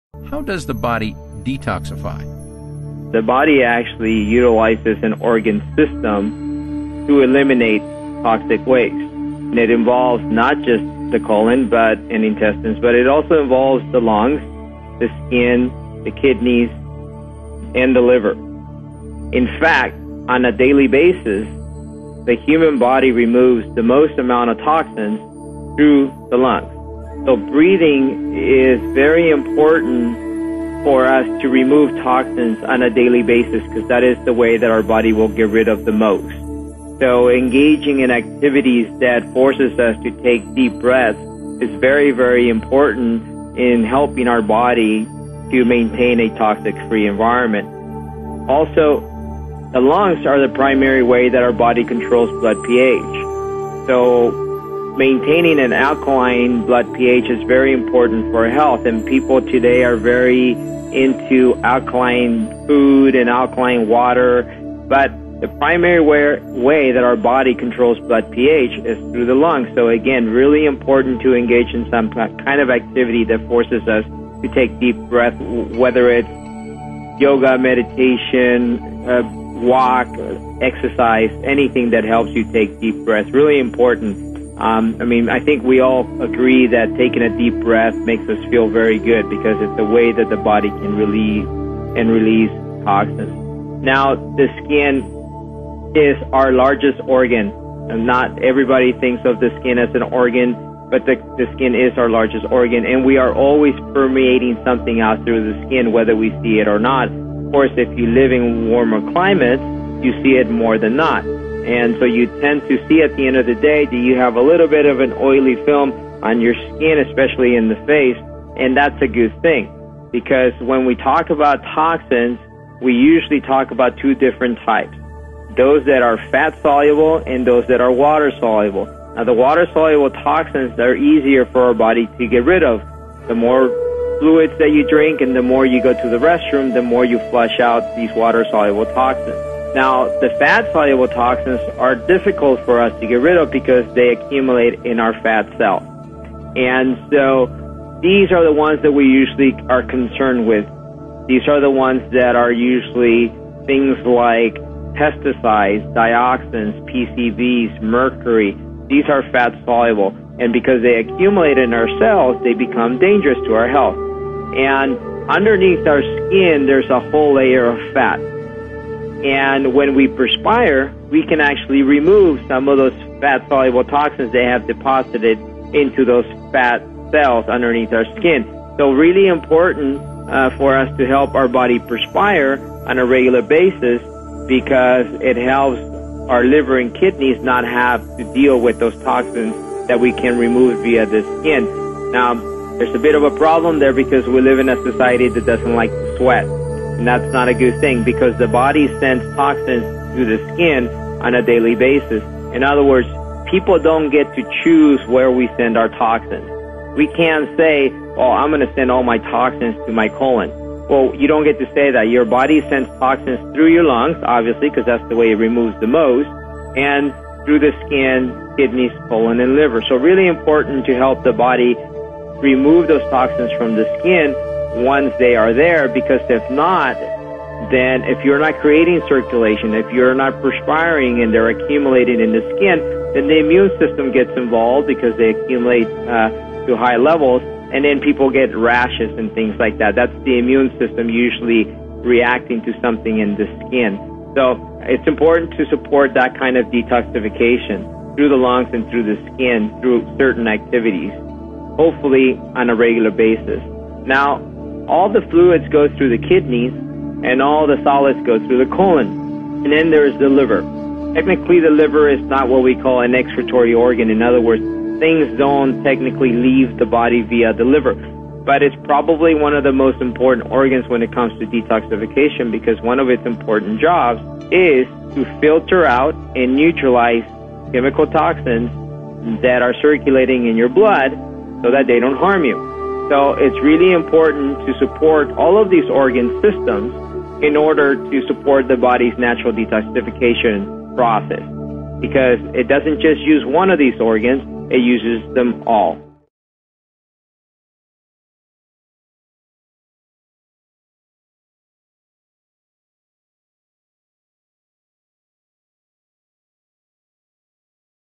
How Does the Body Detoxify?
The body doesn't use just one of the organs, it uses them all.